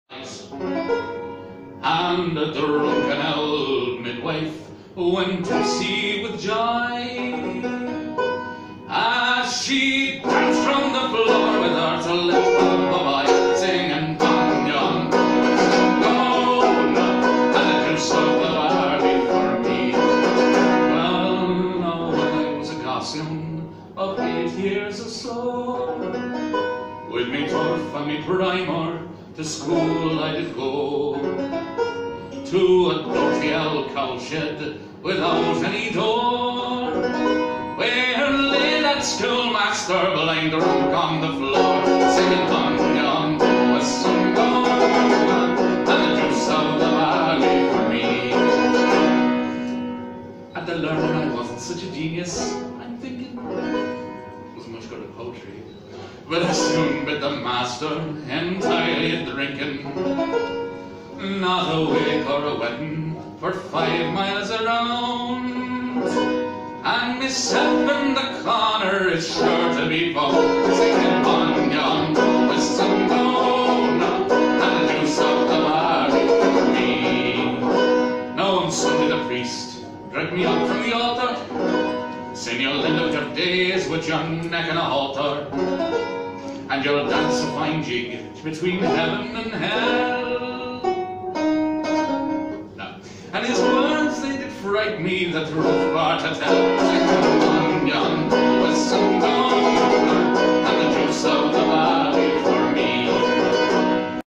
Jack Scrimshaw starts the evening off 09.04.19.
Jack has such a strong voice and an excellent musician for those that don't know he is visually challenged everything is from the heart. I always feel if someone as good as Jack starts us off it sets the bar for the rest of the night.